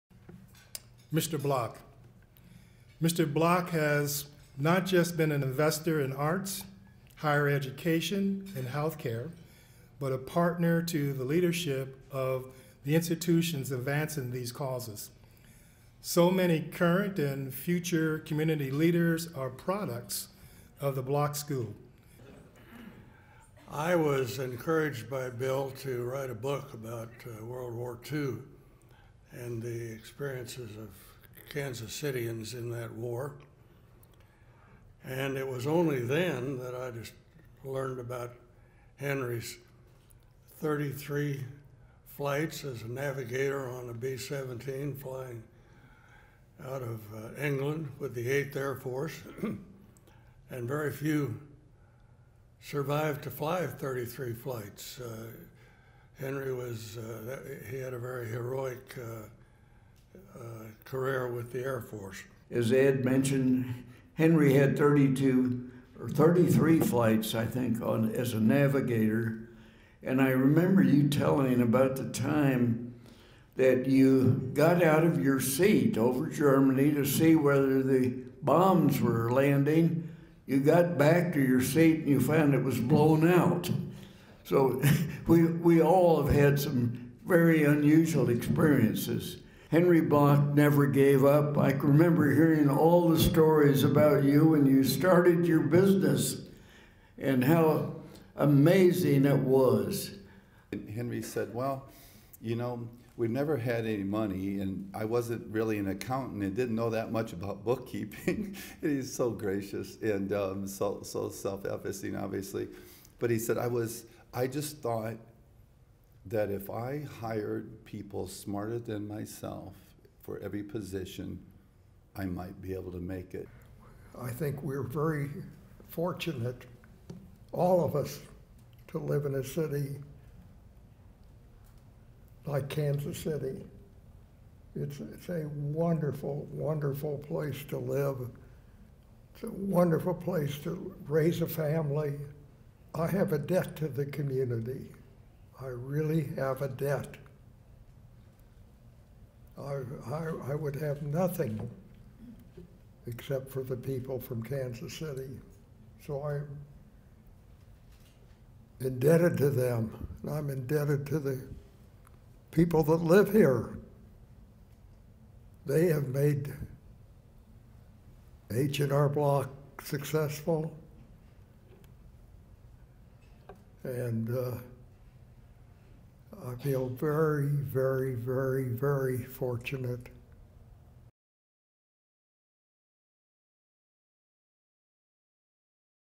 Henry Bloch Tribute 2017
In 2017, Ingram's named Henry Bloch Philanthropist of the Year. During a celebration of the award, a several speakers paid tribute, including colleagues and friends. The H&R Block and continuous giver to the community, told the audience that he couldn't have done it without the support of Kansas City citizens.